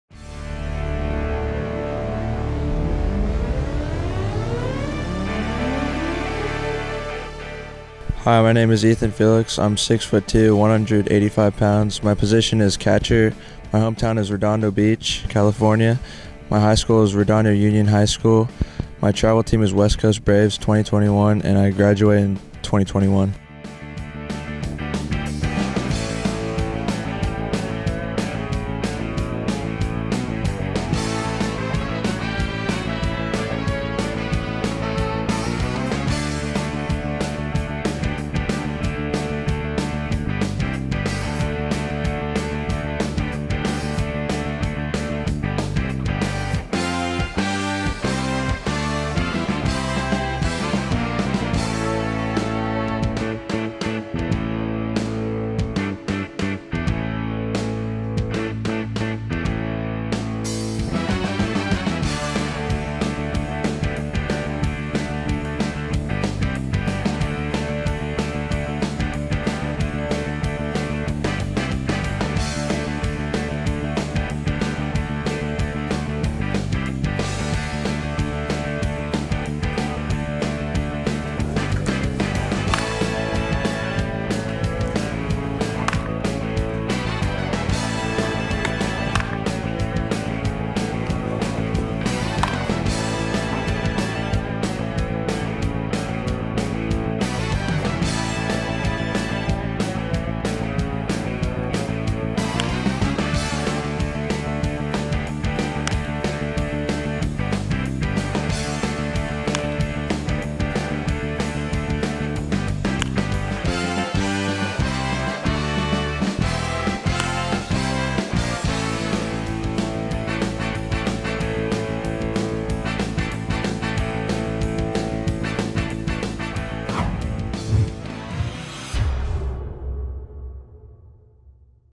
Ethan Felix - C - Redondo Beach, CA - 2021
Perfect Game California Prospect Showcase filmed by SkillShow. East LA College. Monterey Park, CA. November 2 - 3, 2019.